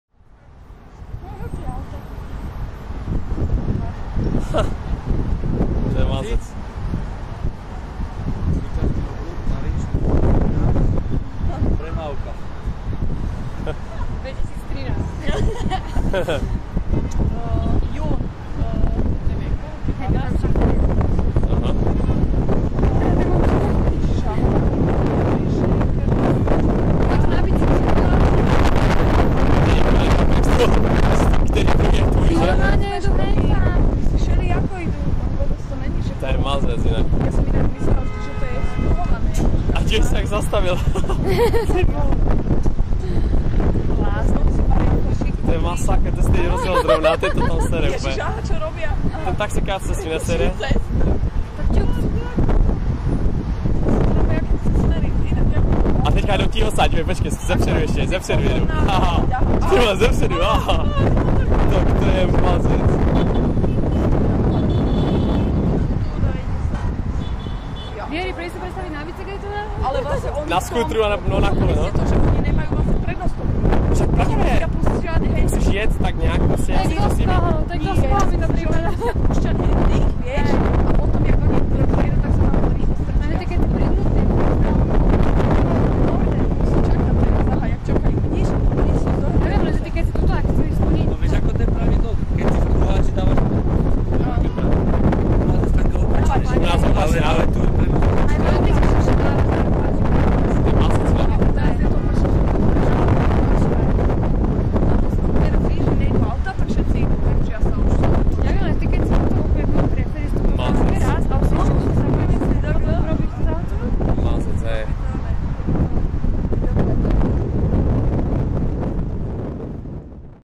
The Craziest Roundabout in Europe - Arc de Triomphe in Paris
This is probably the craziest roundabout in Europe. At the first look, you may think this is somewhere in India... But actually, this roundabout is in Paris. It's Arc de Triomphe roundabout! It's insane!